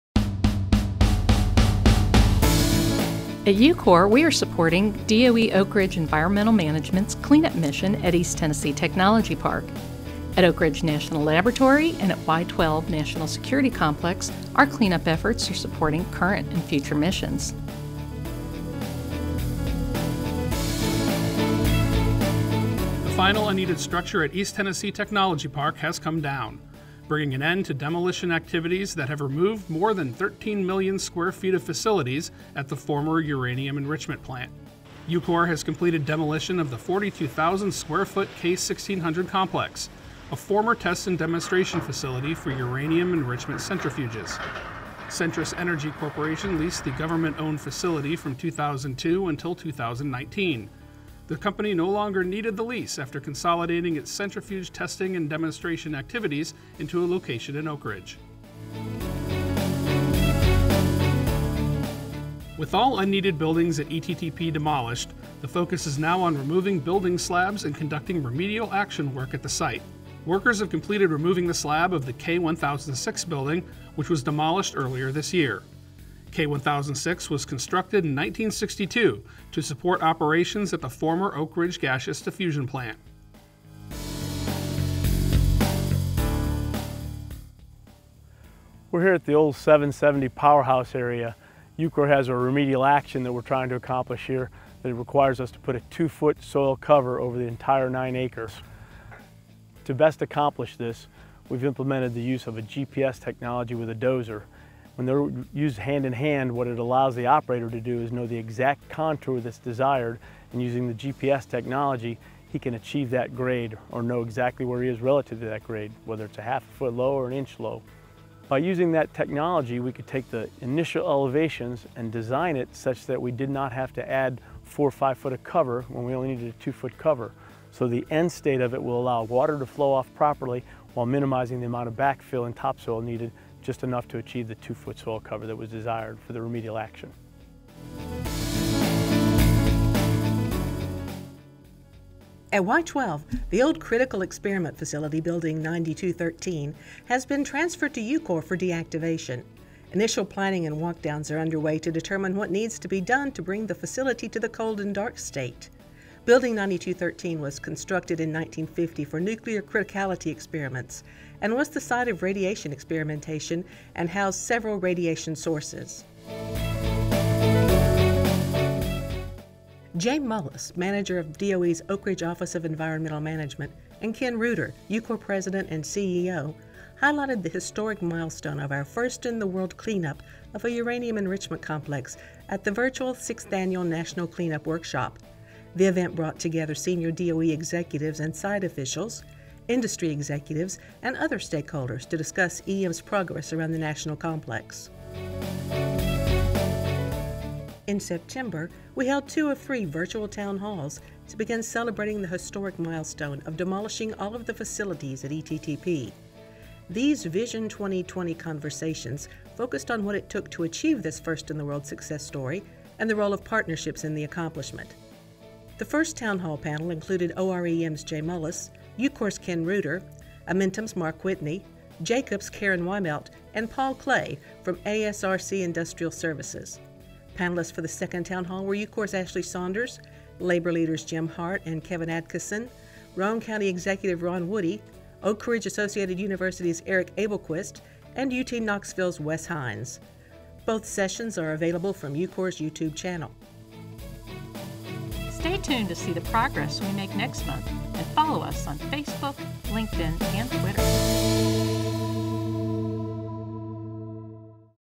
September 2020 Cleanup Progress
UCOR and the U.S. Department of Energy's Oak Ridge Office of Environmental Management made great progress in September. At ETTP, demolition was completed on the last major facility, drawing DOE’s Vision 2020 to a close. At Y-12, Building 9213 has been transferred to the EM program. OREM and UCOR leaders presented at the National Cleanup Workshop and participated in virtual town halls celebrating the successful completion of Vision 2020.